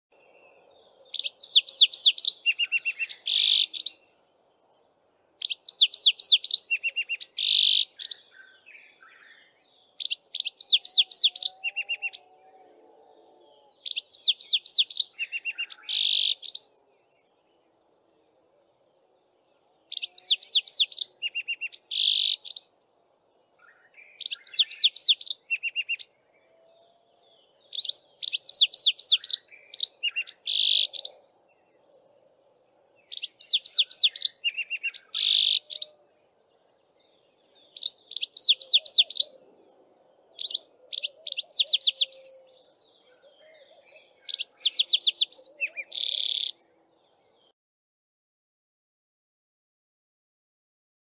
Streaky-headed Seedeater male singing beautifully
Listen to this beautiful song of the Streaky-headed Seedeater, singing in true canary style from the top of a bush in the dawn chorus. Males usually sing like this when the females are busy with nest building.